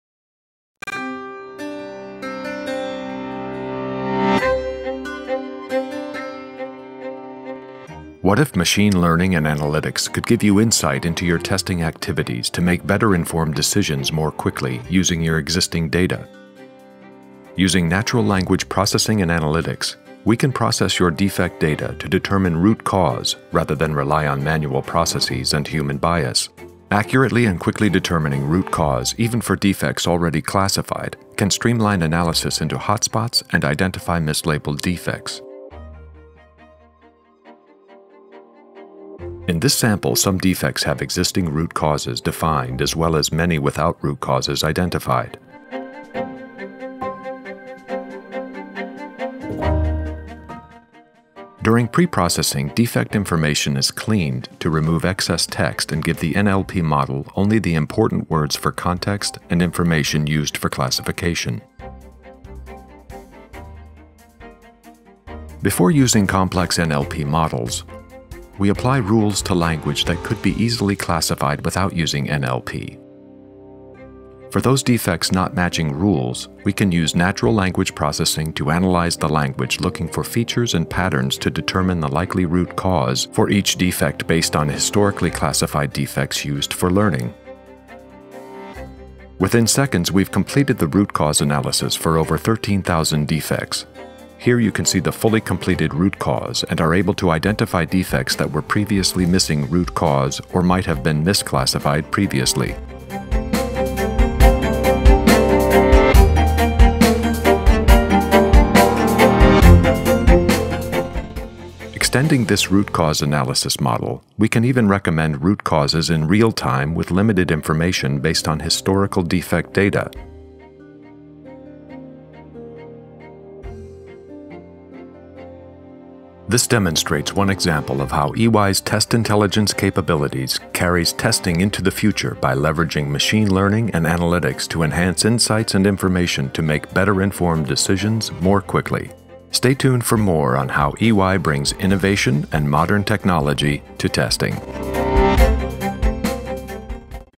Test Intelligence demo: Defect Root Cause Analysis using NLP
Watch how we use machine learning to automatically predict defect root cause analysis - just another way that EY is bringing Testing into the future.